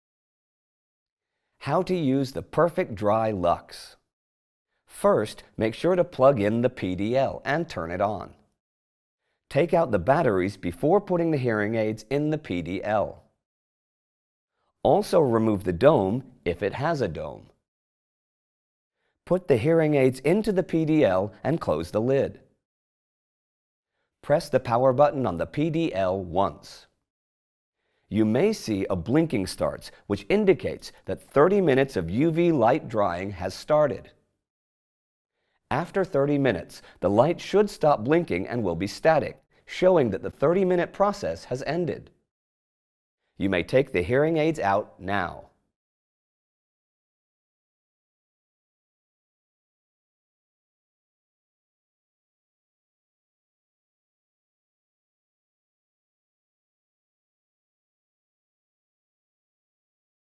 How to use the PerfectDry LUX | hear.com
With the PerfectDry LUX, you can store, dehumidify and clean your hearing aids. Watch the video and learn how to use it with hear.com

1. First, make sure to plug in the PDL, and turn it on.
2. Take the hearing aids.
3. Take out the batteries before putting the hearing aids in the PDL.
4. Also remove the dome if it has a dome.
5. Put the hearing aids into the PDL and close the lid.
6. Press the power button on the PDL once.
7. You may see a blinking starts, which indicates that 30 minutes of U.V Light drying has started.
8. After 30 minutes, the light should stop blinking and will be static, showing that the 30-minute process has ended.
9. You may take the hearing aids out now.